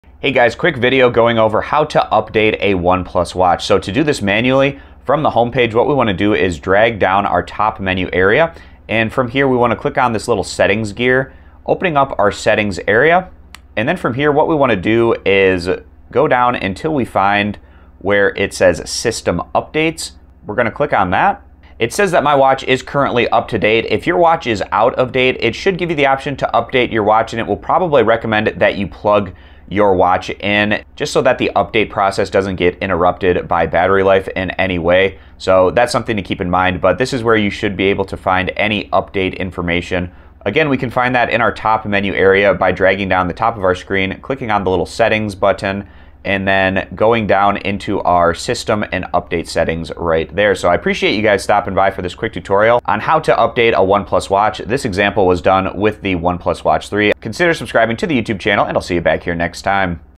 OnePlus Watch How To Update - How To Software Update One Plus Watch 3 Smartwatch
One Plus Watches on Amazon

Hello, In this video I talk about OnePlus Watch How To Update - How To Software Update One Plus Watch 3 Smartwatch Guide Step by Step Tutorial, Guide, Example, Instructions, Video Help

In this video I walk through a simple and easy step by step instructions, guide, tutorial on how to update any One Plus Smartwatch such as the OnePlus Watch 3.